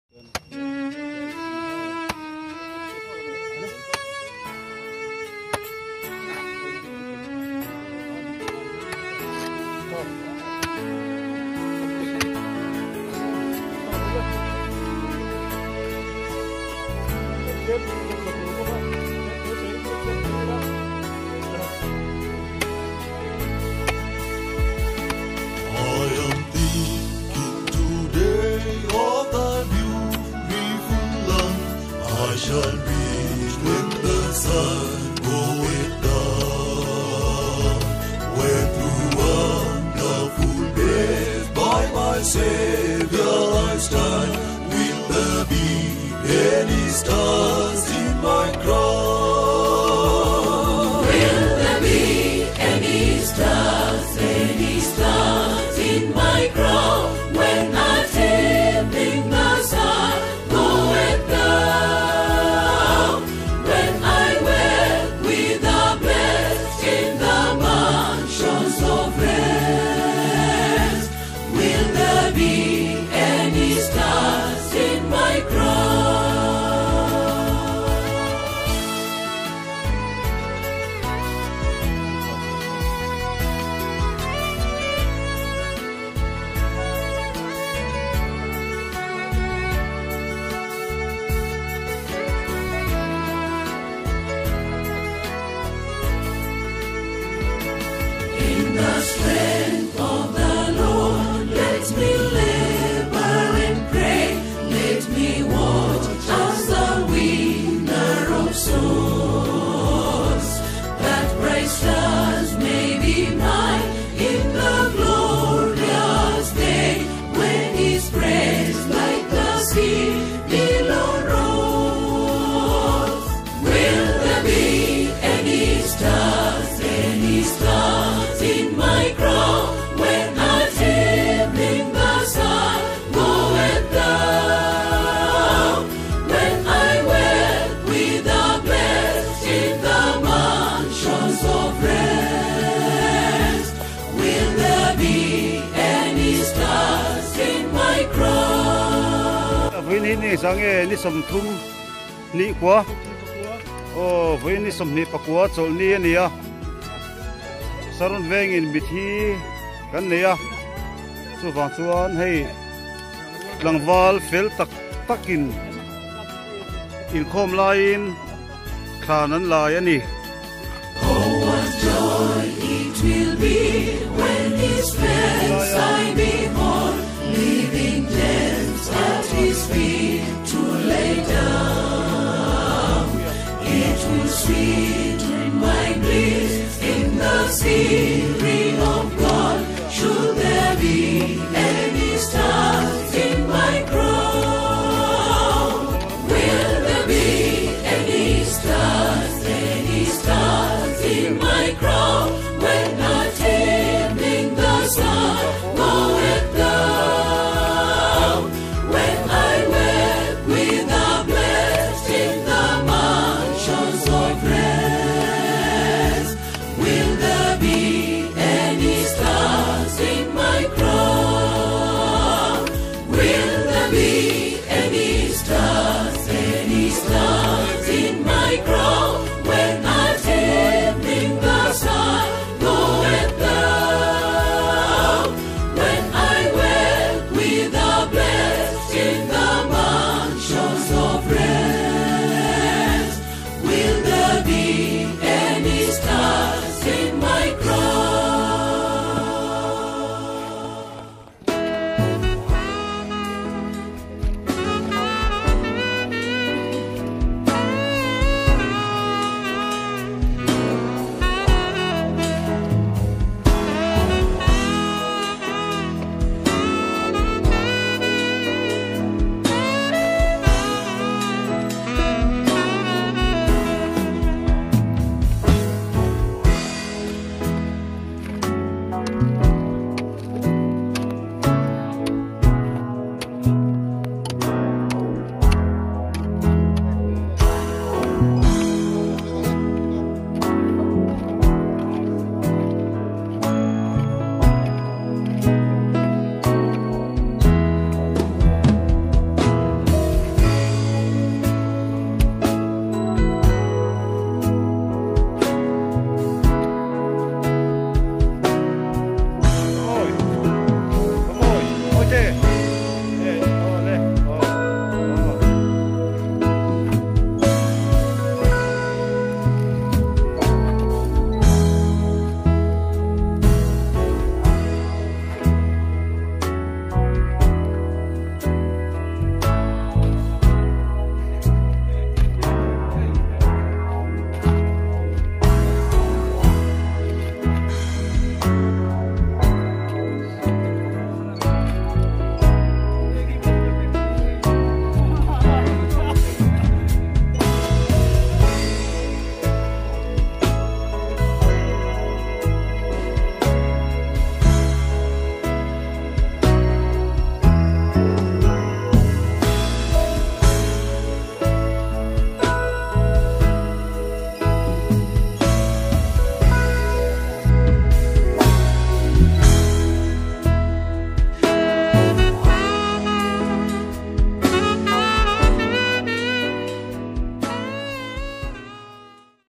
Sunday inkhawm lai a Thlan lai video
1. YMA chuan tanpui ngai te a pui thin. Saron Branch YMA member te inpekna hi a va ropui thin em.
2. Comment section a ka comment hi min lo chhiar sak chuan ka lawm ngawt ang.